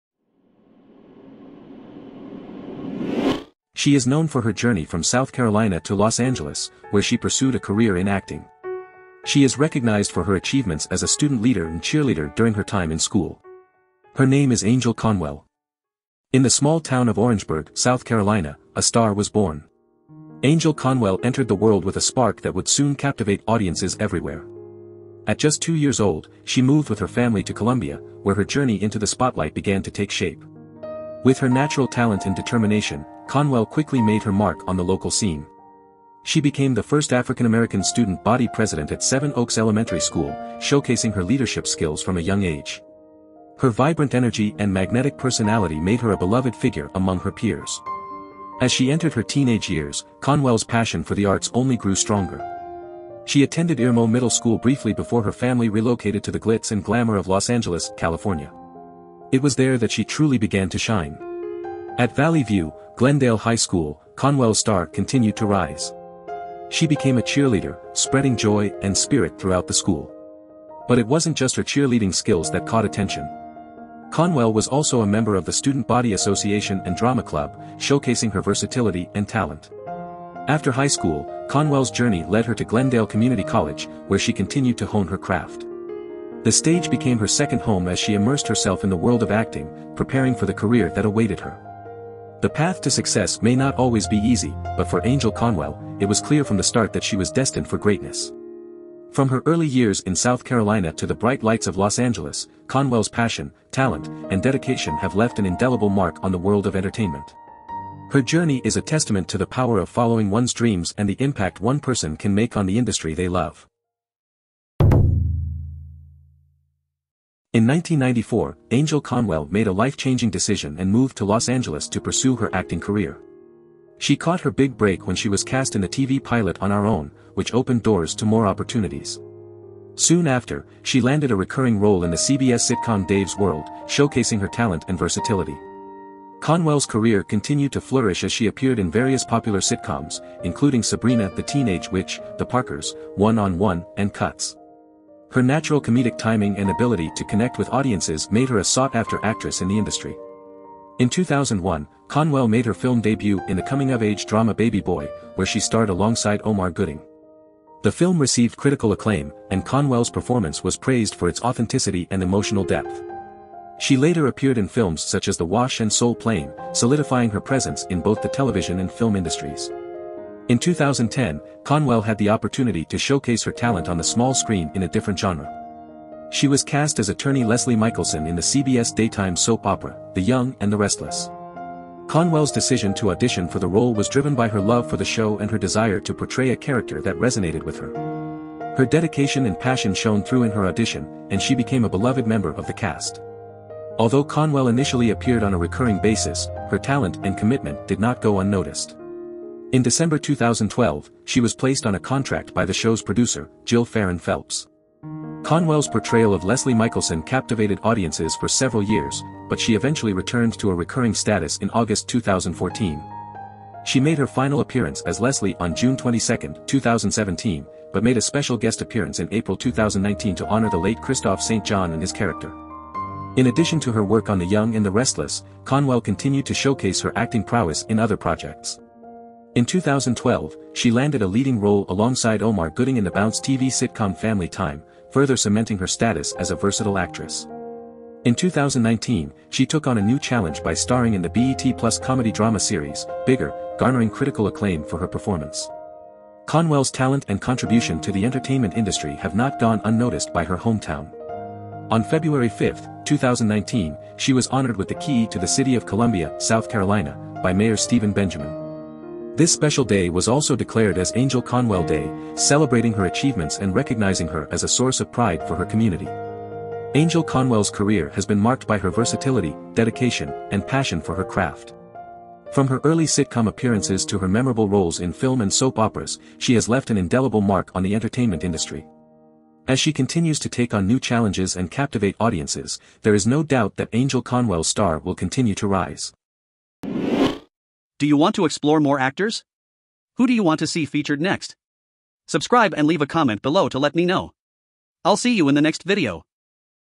Angell Conwell: From South Carolina to Hollywood | Actors & Actresses Biography
In 1994, Conwell moved to Los Angeles to film the TV pilot On Our Own. She later was cast in a recurring role in the…